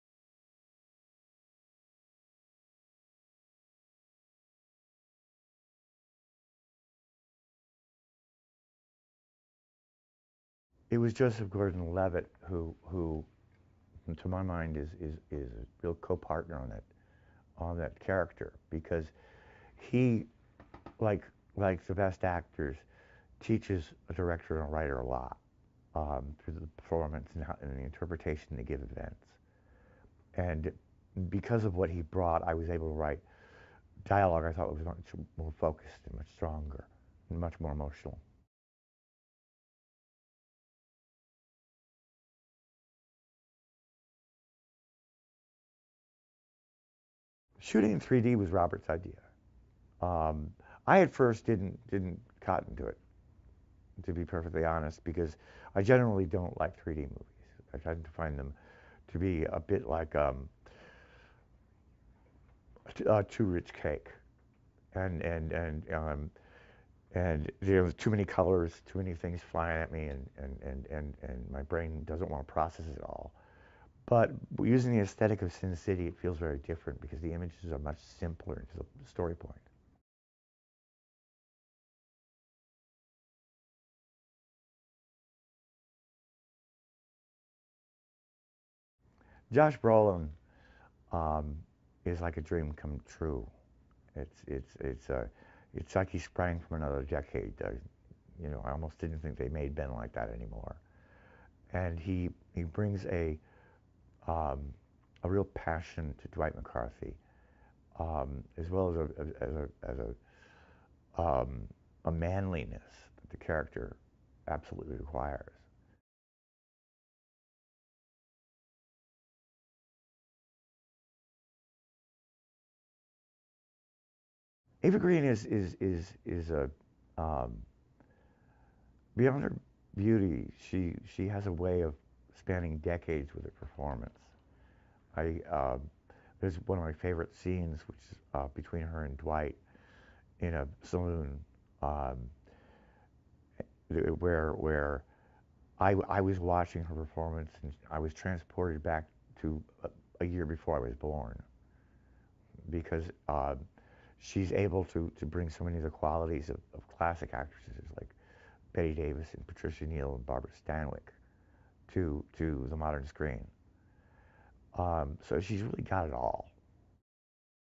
Sin City: A Dame To Kill For - Interview - Frank Miller
Interview with Frank Miller (Writer) for Sin City: A Dame to Kill For.